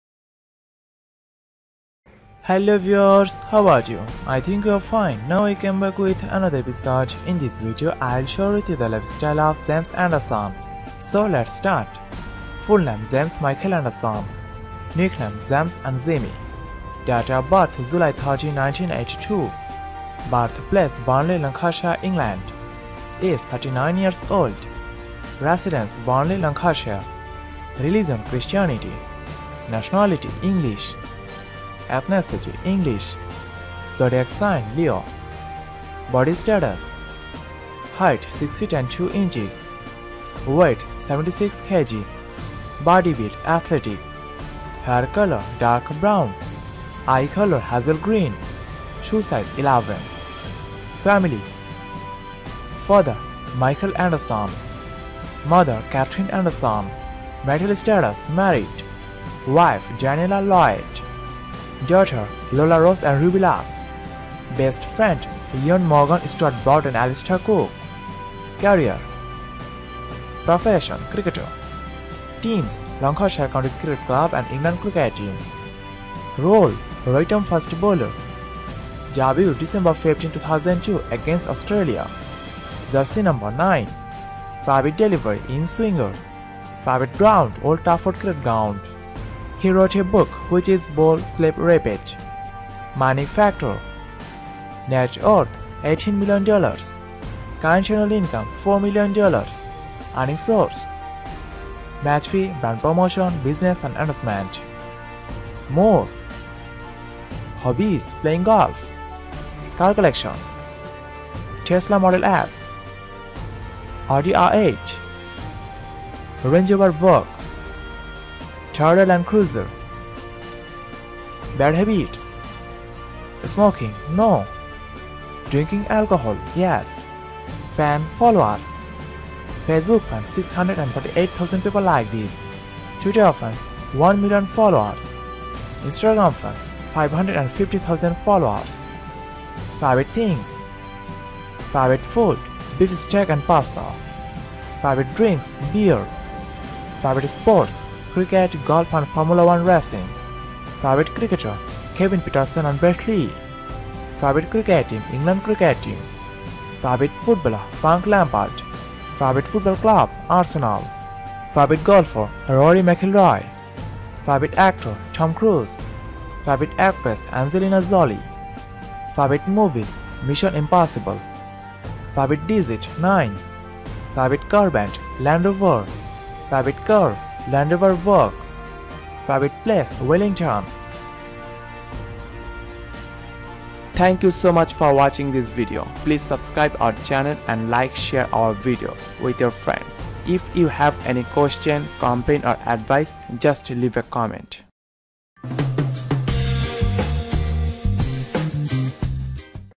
James Anderson Lifestyle 2022 ★ James Anderson ★ Top 10 Series Pro
James Anderson is an English cricketer who plays international cricket for England Cricket Team as a right-arm fast bowler.

Jimmy made his debut for England in December 2002 against Australia. Currently, he is the number 8th in the test bowling rankings according to the ICC. Anderson is the all-time leading wicket-taker among fast bowlers and he holds the record of most wickets for England in Test and ODI formats of cricket.

Jimmy Anderson is the first fast bowler as well as the first English bowler and the fourth overall to pass 600 Test wickets.

In this video, we have included James Anderson's lifestyle, biography, family, annual income, net worth, girlfriends, car collection, and favorite things also. So, Enjoy!!!

Title - James Anderson Lifestyle 2022 ★ James Anderson ★ Top 10 Series Pro

If have any question, complain or advice, please Comment.